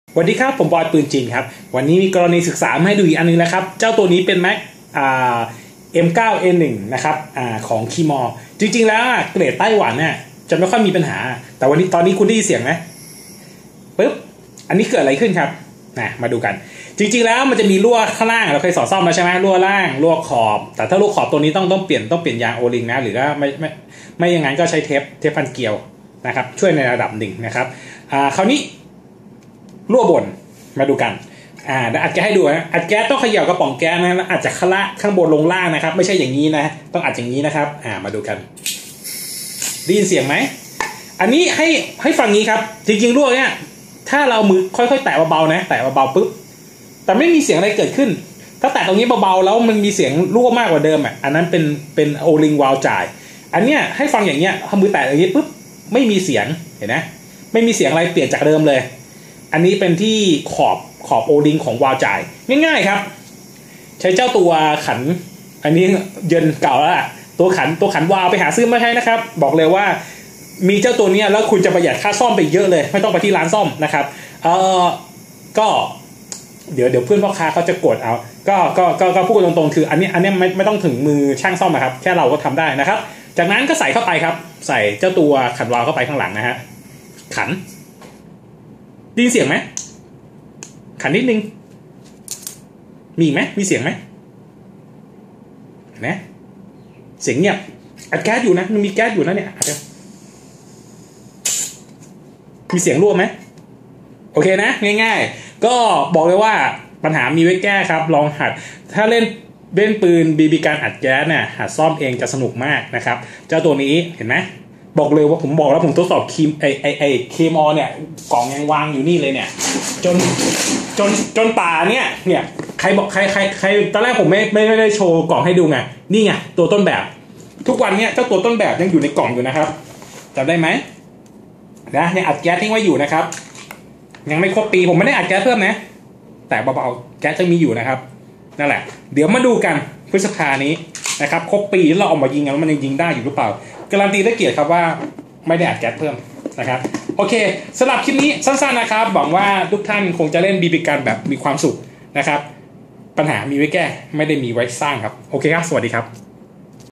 แมก keymore ซึมจากด้านบน ทำไง
คร่าวๆนะครับ ไม่ยาก คุณก็ทำได้ ขอจงเล่น bbgun อย่างมีความสุข ปัญหา พวกผมแก้เอง